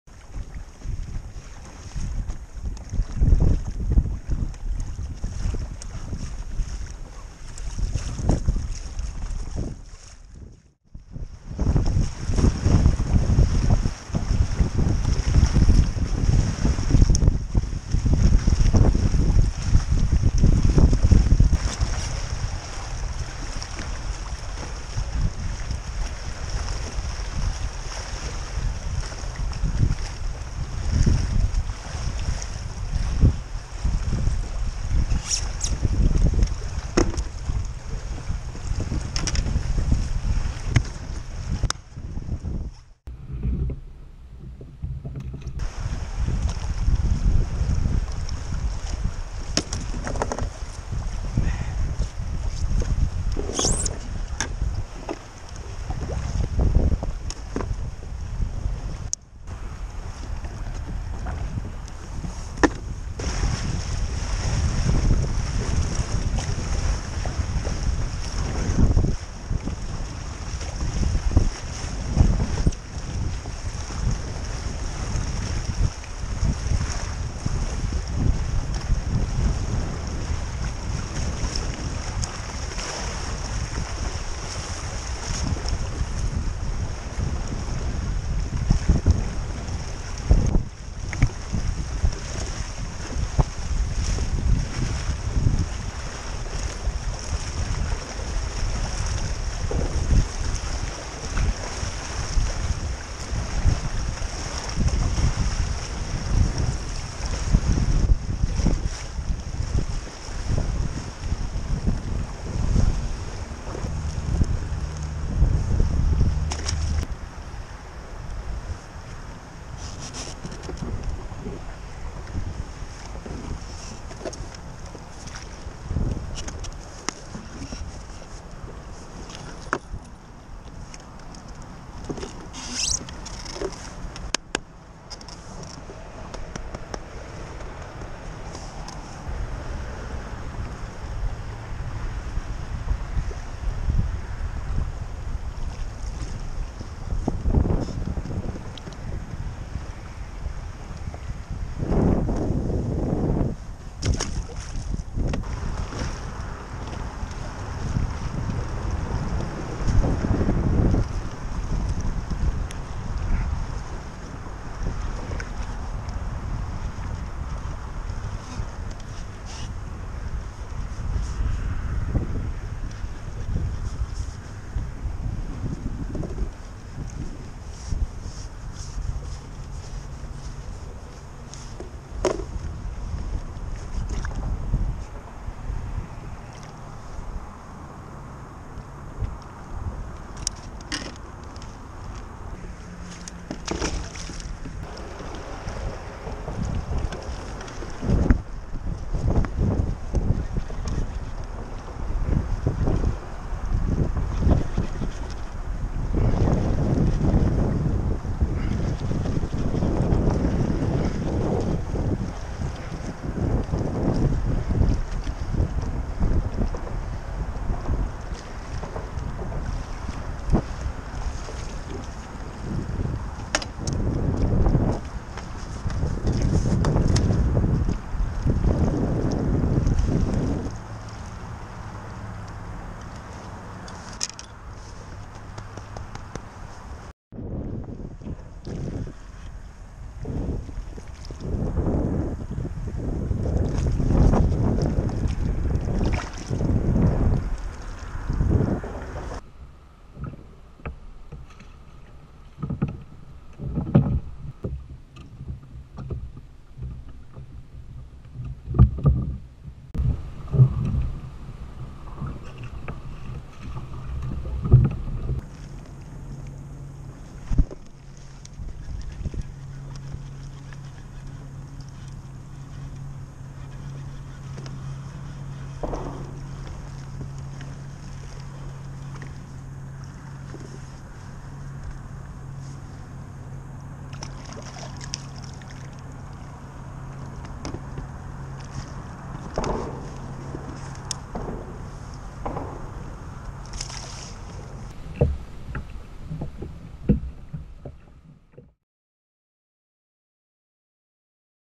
Tautog at Chesapeake Bay Bridge Tunnel, VA (Apr 4, 2021)
Kayak fishing for tautog at CBBT (Chesapeake Bay Bridge Tunnel), VA
Bait: shrimp
Water Temp: 52-54 F
Caught 3 keeper and 15 undersized Tautog